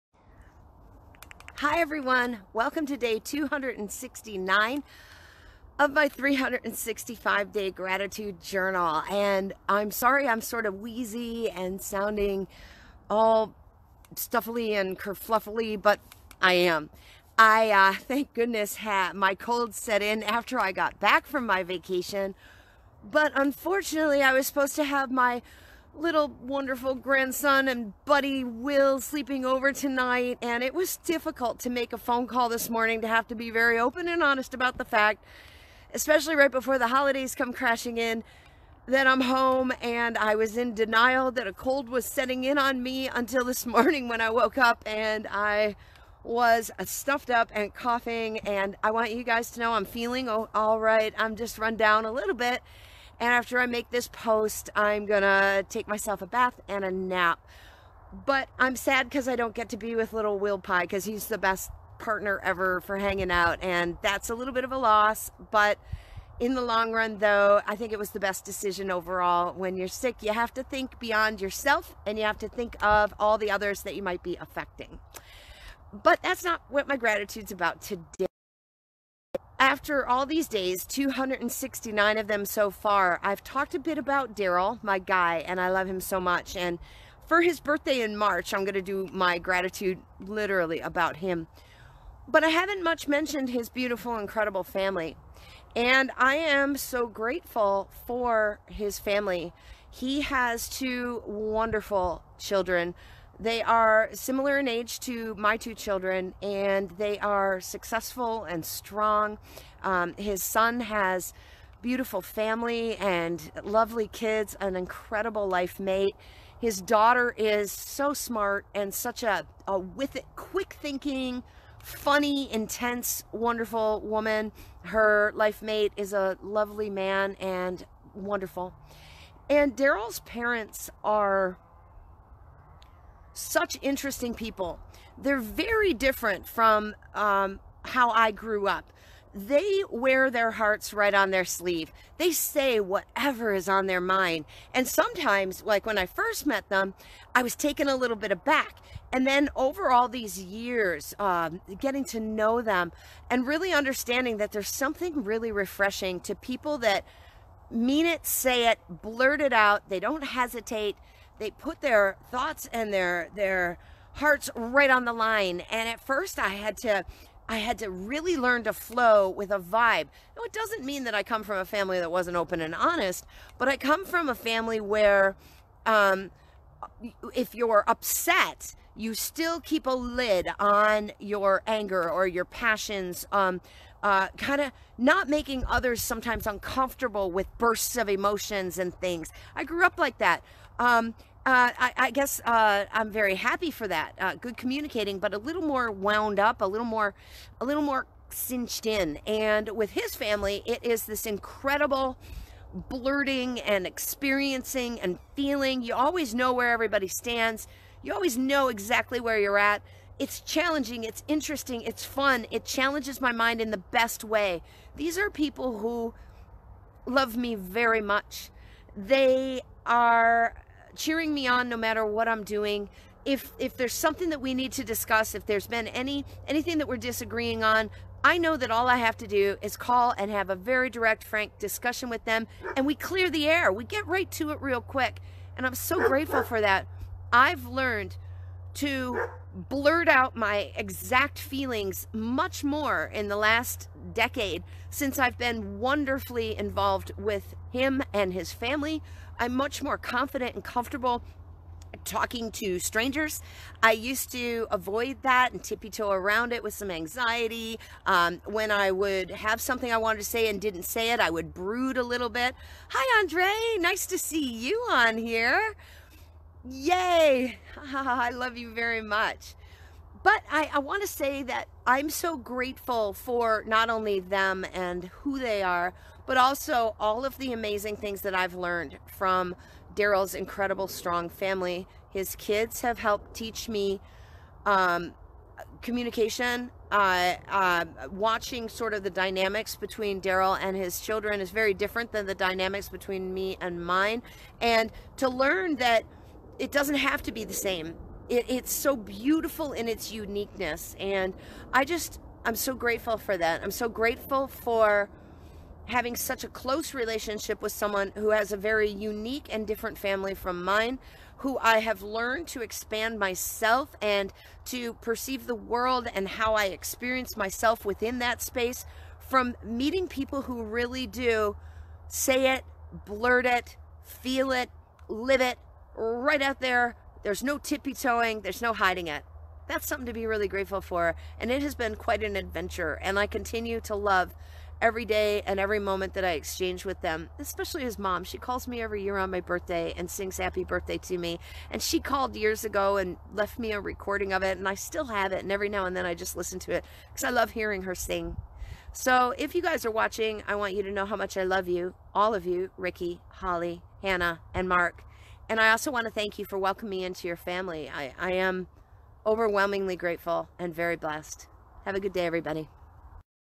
Grateful For In-Laws & Step-Kids
Day #269 of 365 Daily Gratitude -
I'm up on top of my shed again today. Grateful for my extended family through my partner Daryl. His family is beautifully different from my own and how I was raised. I have grown and expanded in many ways since getting to know and love them. I remain thankful and ever intrigued, challenged and wise due to their influence in my life. Wowzers! Not only does he rock... but they take it all up a notch even more! Thank you.